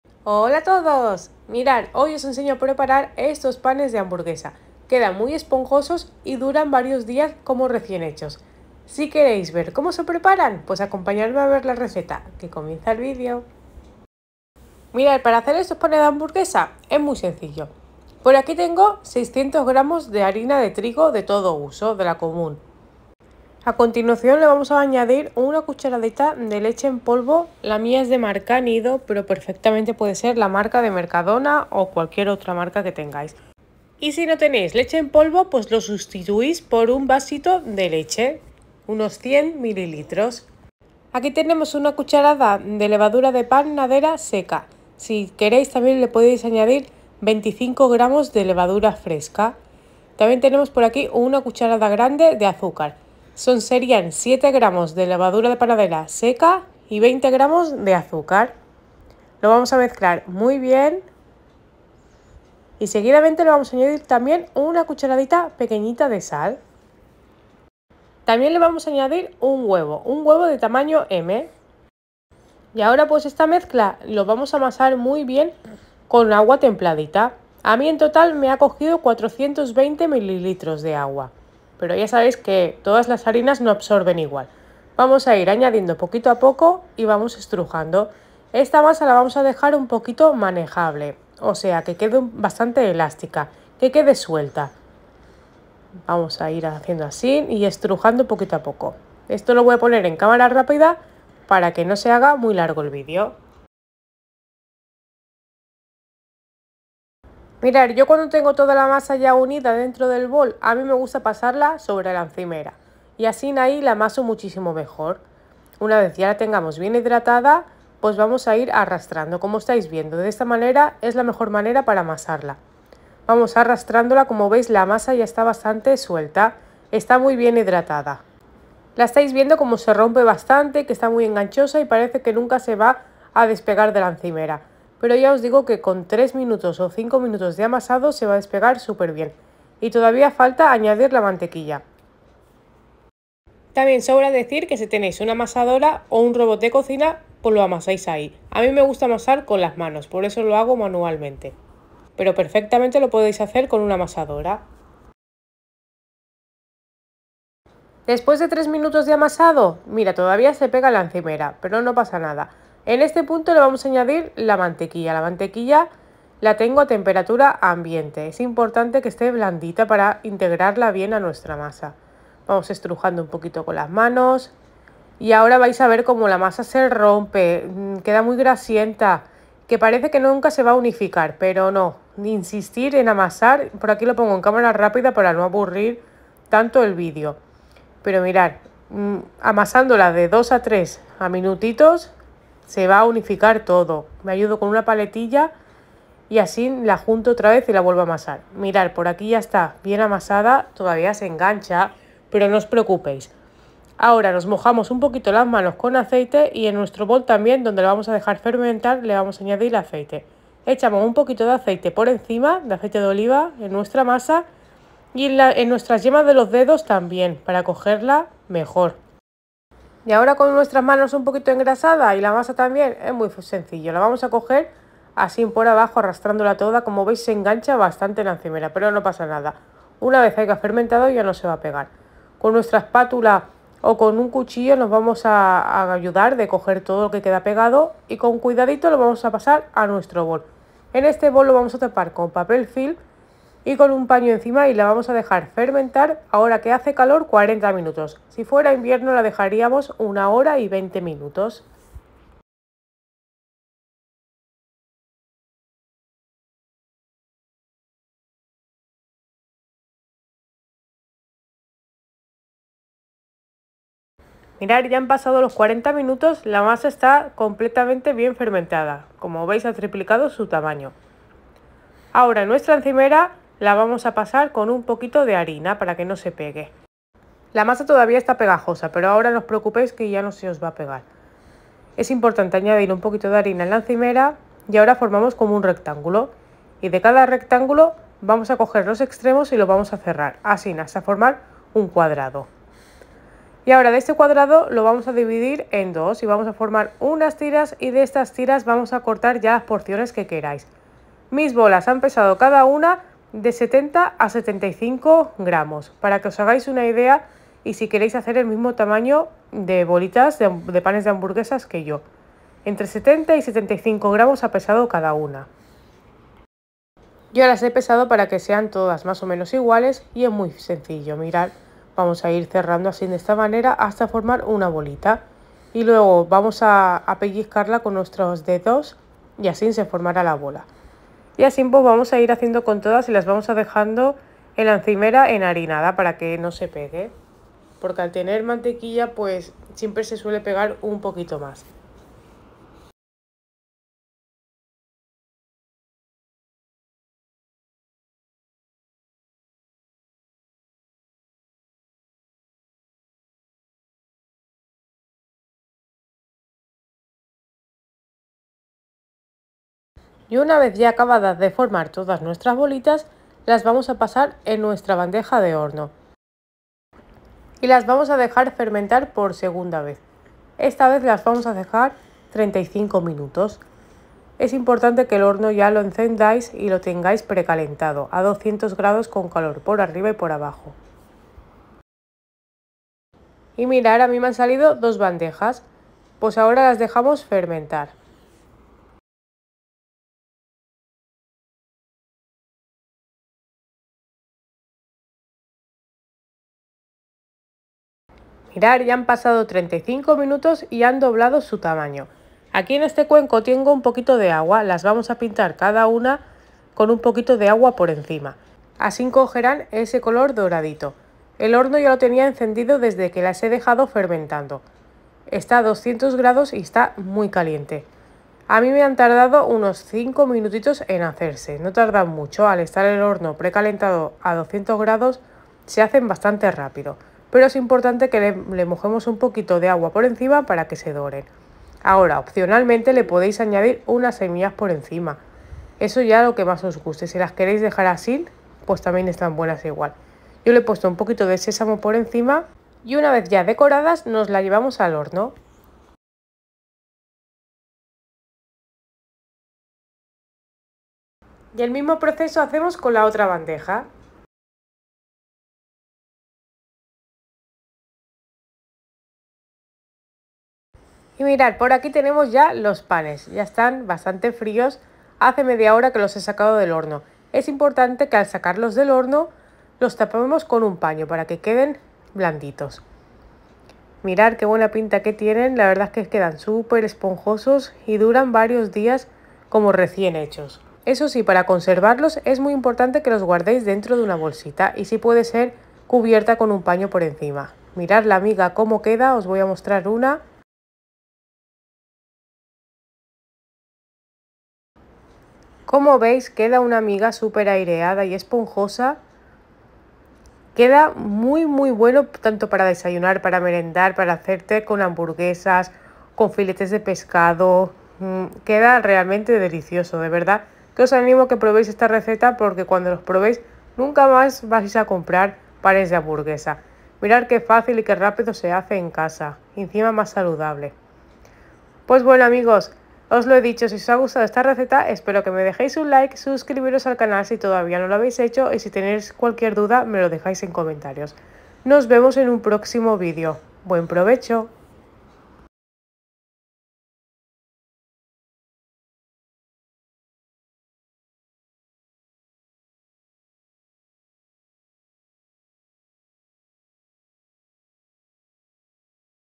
Bollitos o panes de hamburguesa muy esponjosos y dura varios días como recién echo‼️
Hola a Todos! Por aquí abajo os dejo los ingredientes escritos para que os sea más fácil prepararlos😊👇🏼
Ingredientes:
600 gr Harina
1 cucharadita leche en polvo o podéis sustituirlo por un vaso de leche 100ml
1 huevo
70gr mántequilla
1 cucharadita sal
1 cucharada azúcar o 20gr
1 cucharada levadura de panadero seca o 20gr de la fresca
Agua tibia para unir la masa